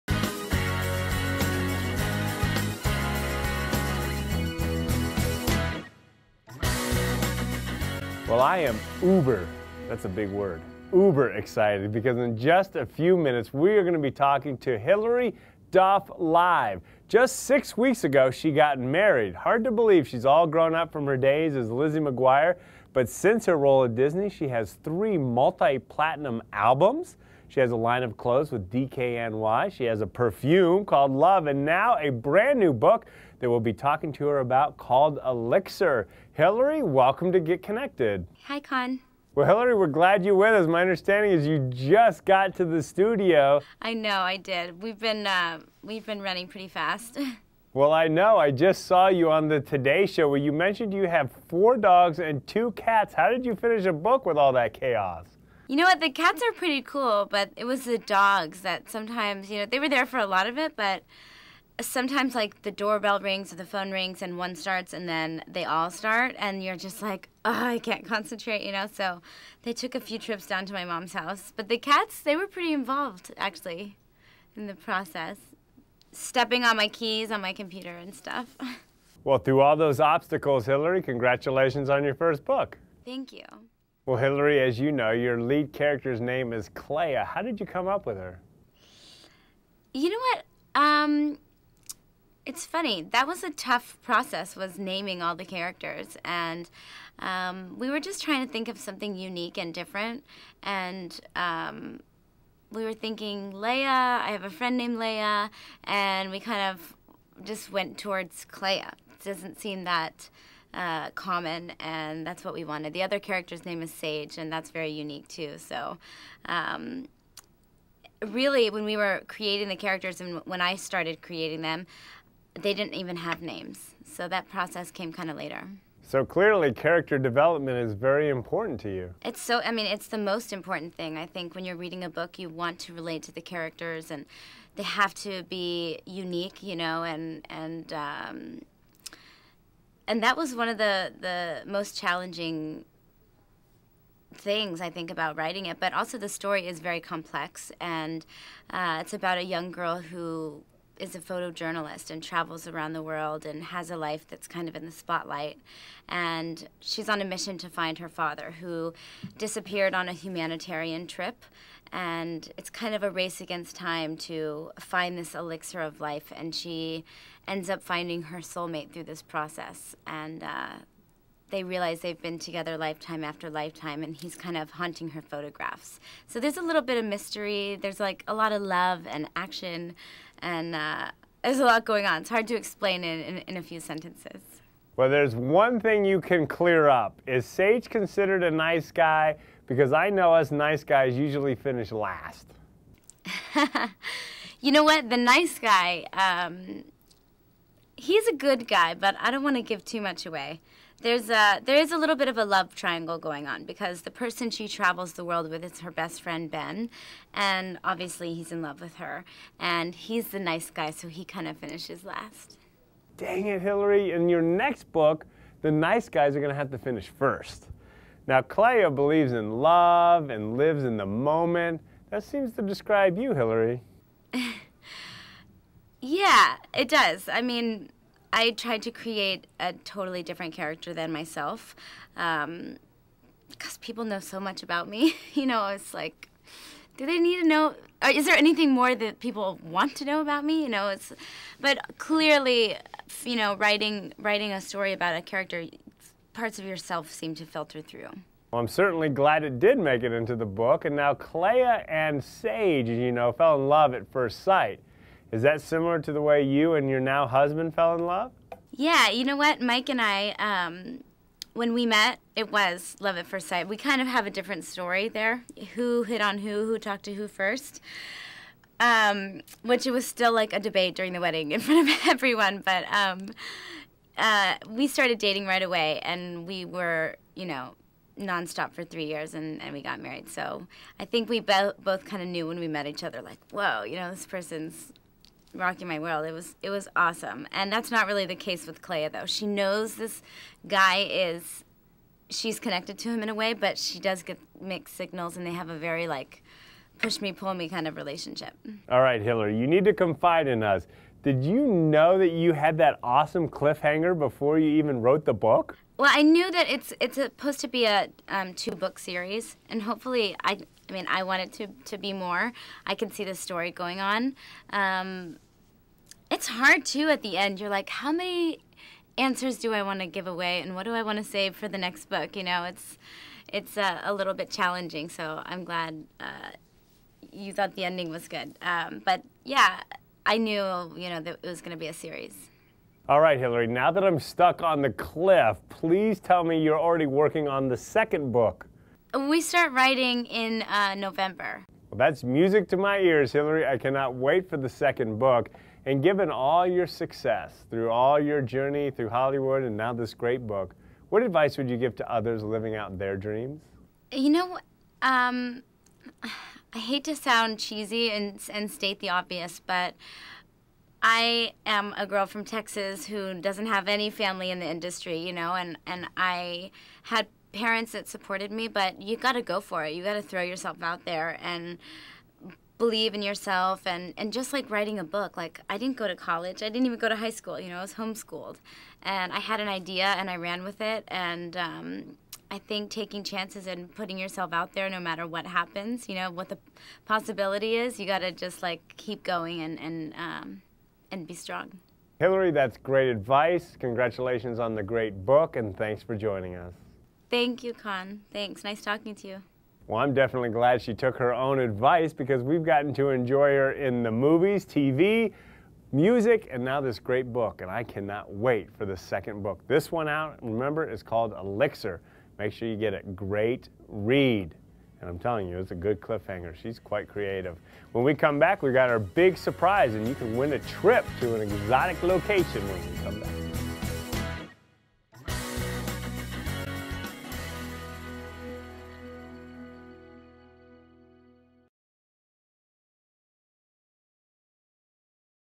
Hilary Duff Interview - Elixir
Watch Conn Jackson as he interviews Hilary Duff live about the release of her new novel, Elixir.